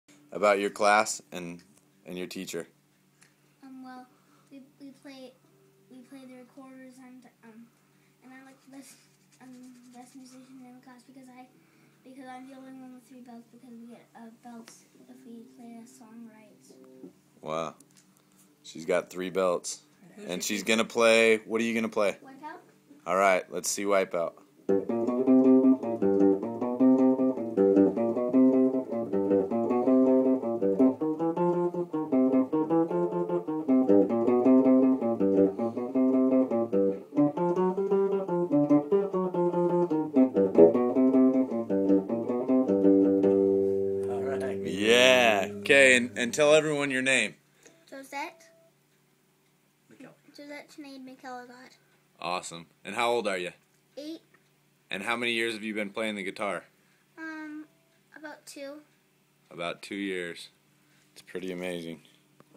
- Josette - aMaZiNG 8 year old guitarist
I met this nice girl and her family as I installed an APX alarm system  in their Oroville, CA  home.  Super talented bunch they are!